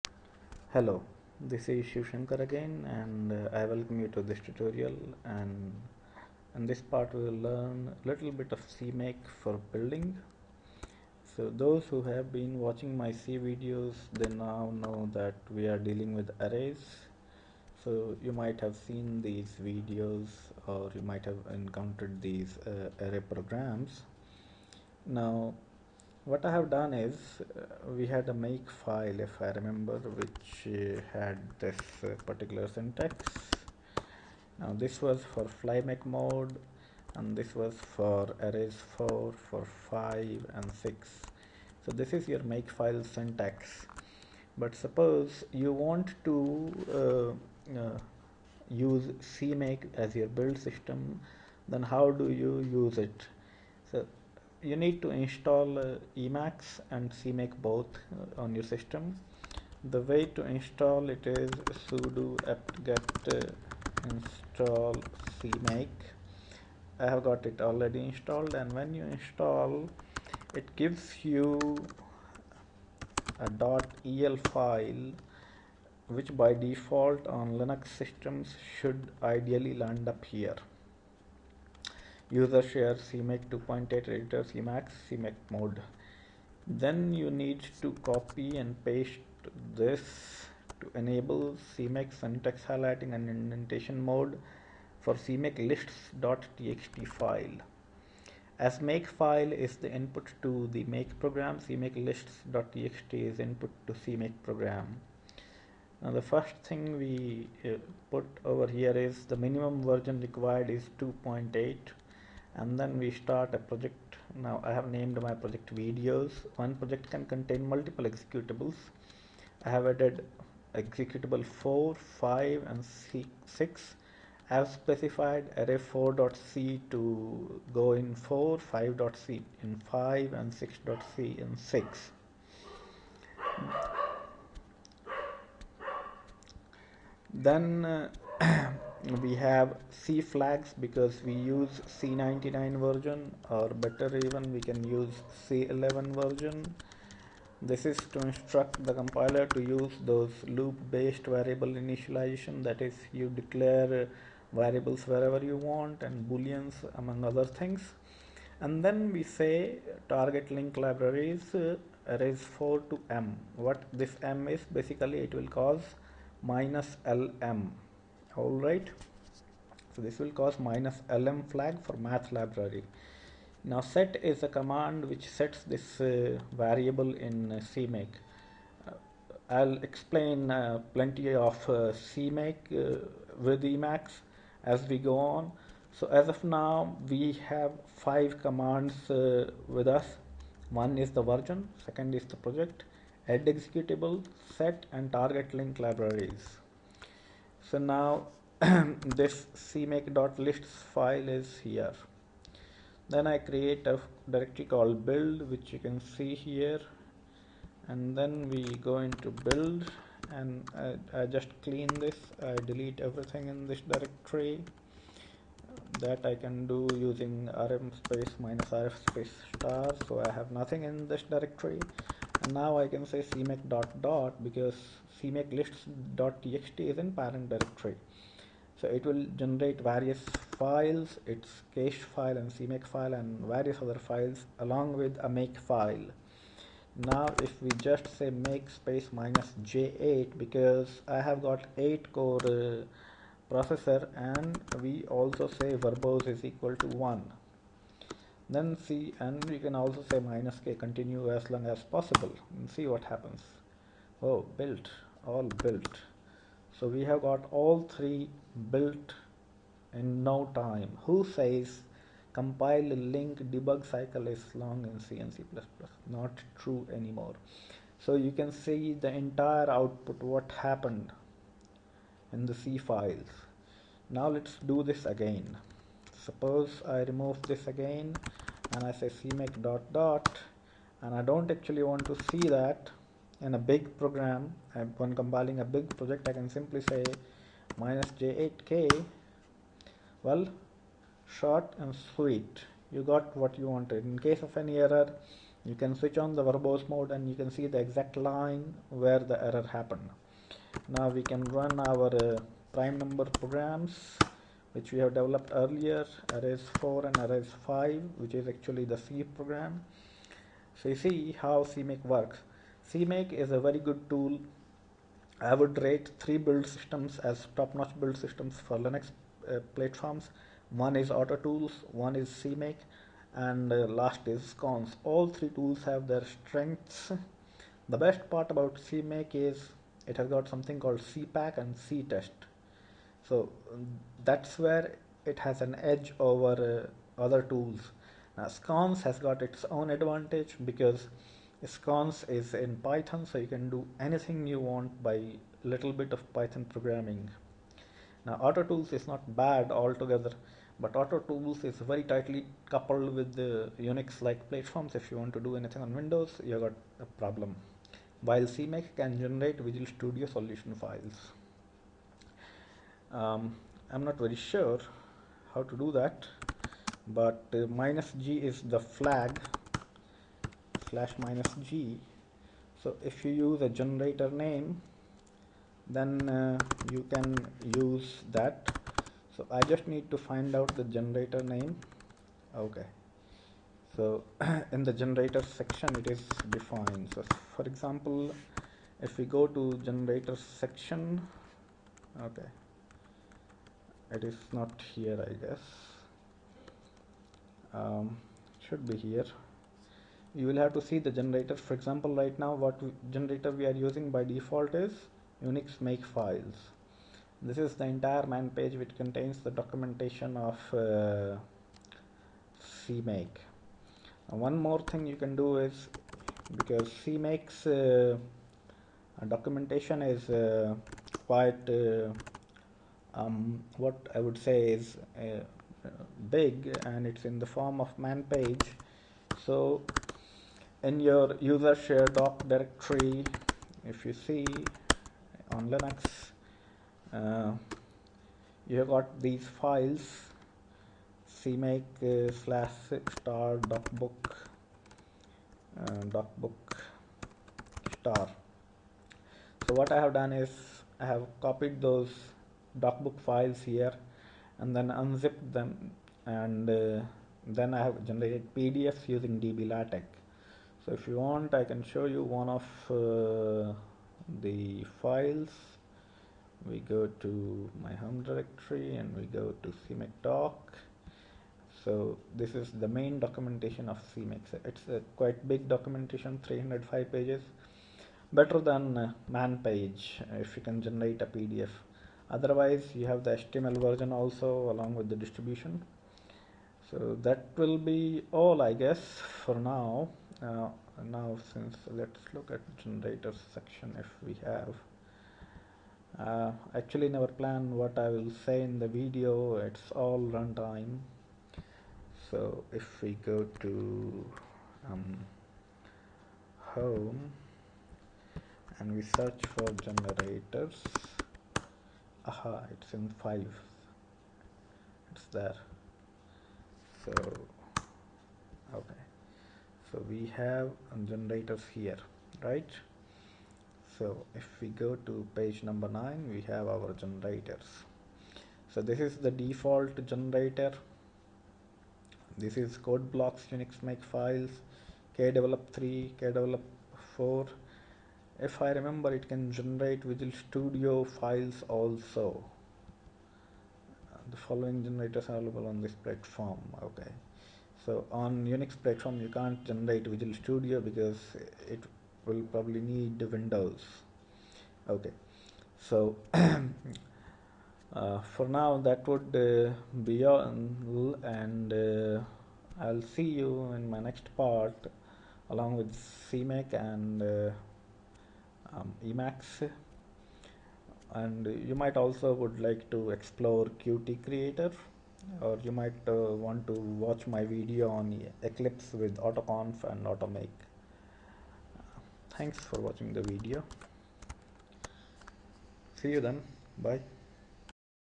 Introduction to Emacs and CMake
First tutorial on how to use CMake with Emacs.